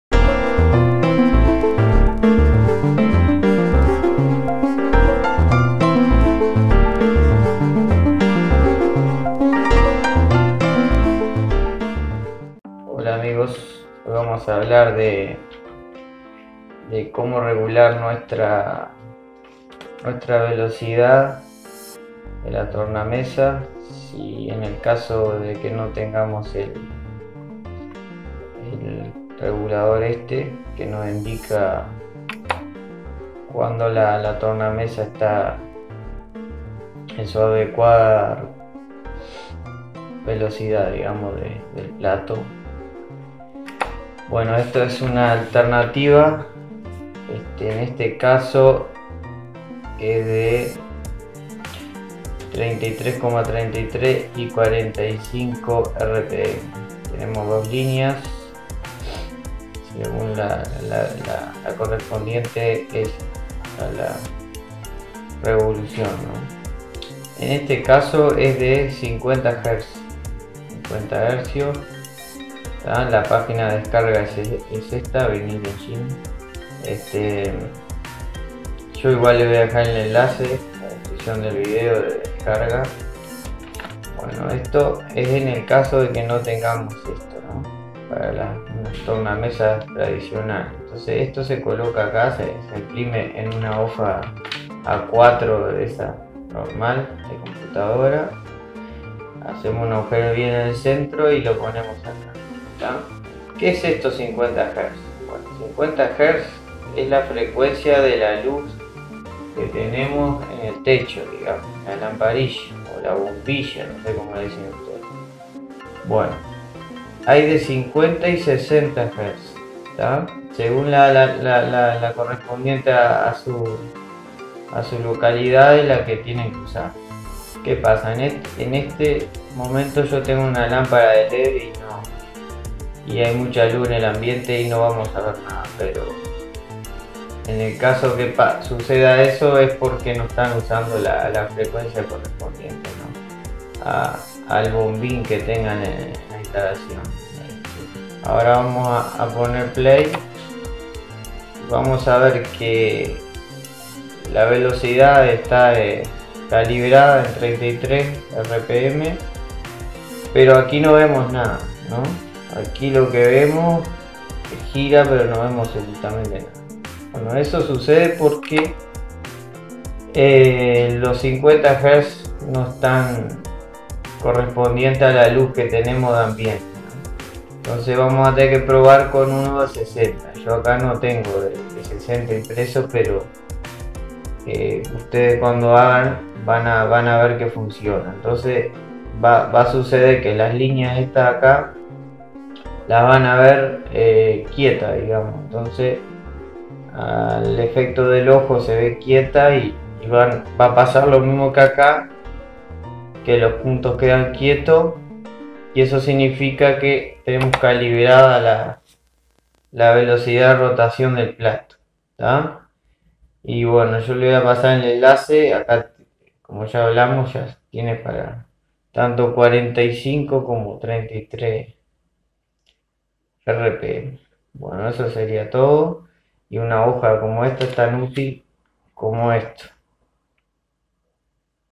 Como calibrar velocidad del plato de la tornamesa (luz estroboscópica - Strobe)
Aquí un pequeño truco de la vieja escuela de como poner nuestra tornamesa a punto en su respectivas estándares de velocidad o revoluciones por minuto (RPM) tanto en 33.3RPM, 45RPM y 78RPM.